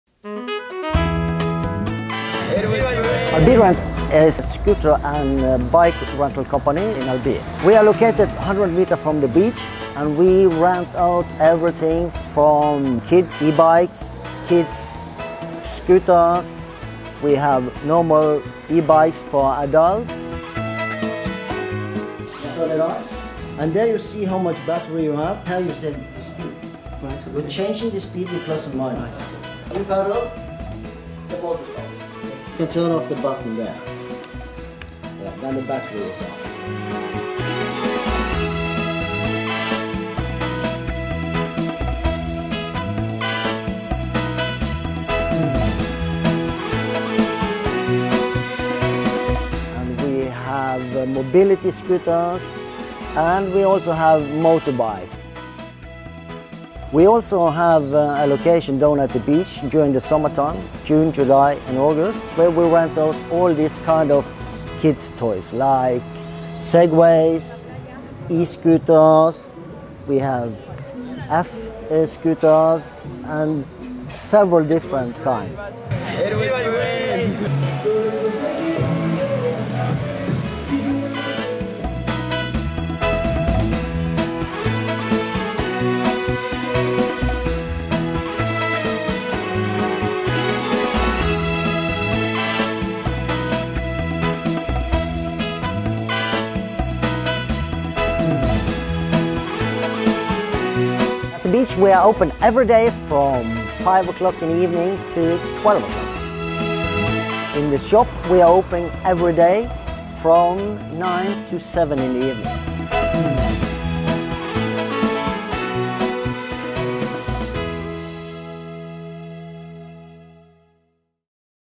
AlbiRent in Albir E-Bike Scooter & Segway
- In this 2-minute film you will explore what AlbiRent has to offer. Rent your E-Bike, Scooter or Segway at AlbiRent which is located 100 meters from the beach in Albir, L'Alfas del Pi in the province of Alicante on the Costa Blanca, Spain.

Film and music created and produced by Theo Newton,